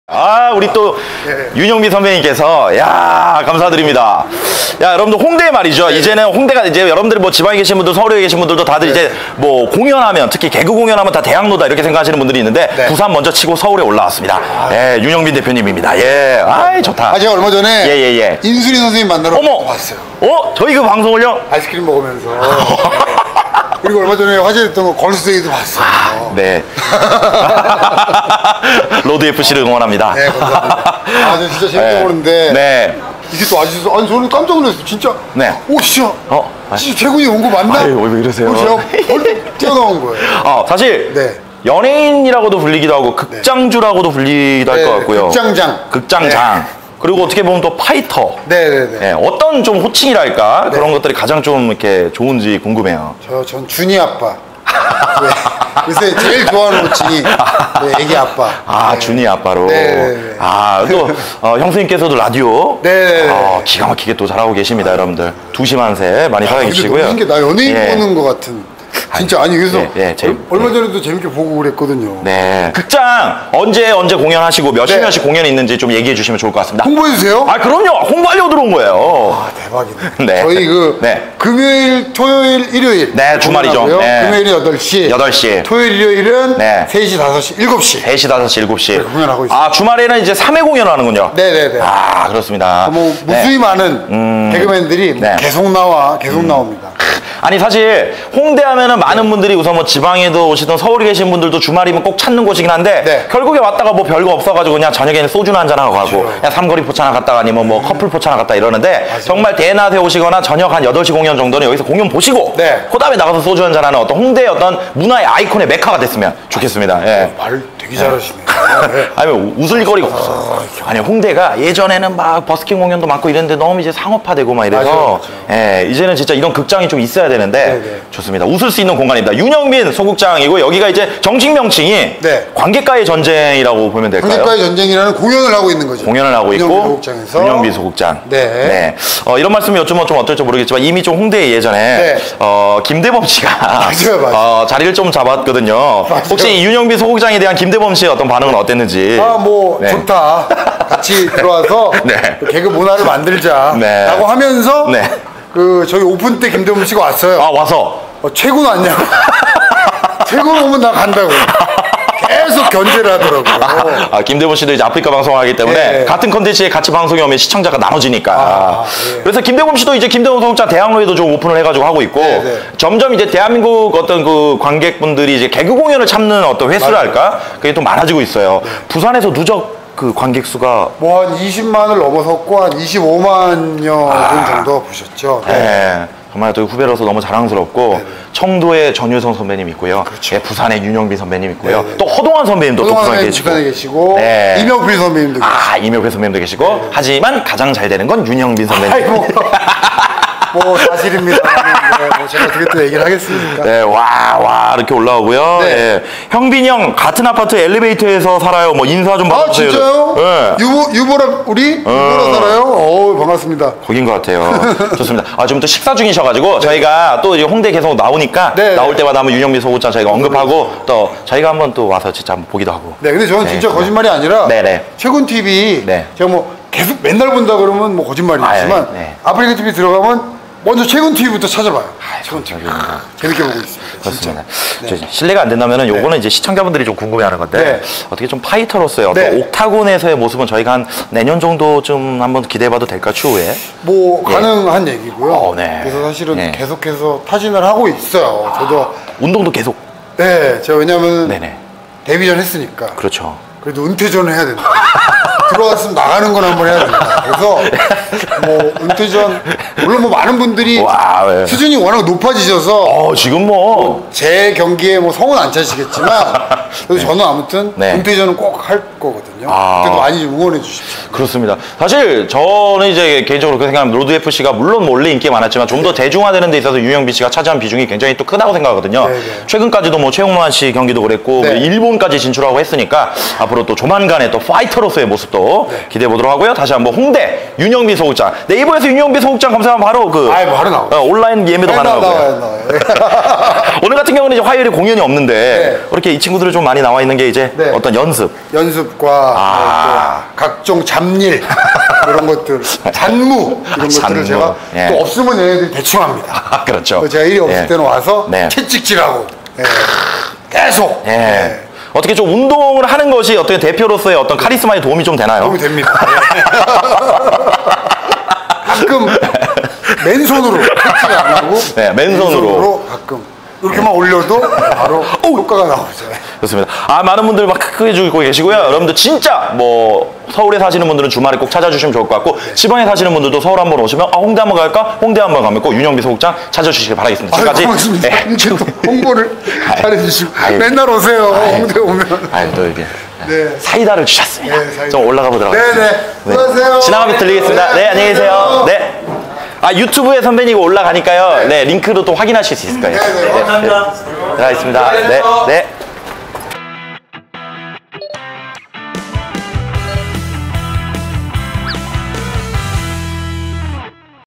개그맨 윤형빈 소극장에서 우연히 만나 근황 인터뷰!! [oh Hot] - KoonTV
개그맨 윤형빈 홍대 소극장에서 우연히 만나 근황 인터뷰!!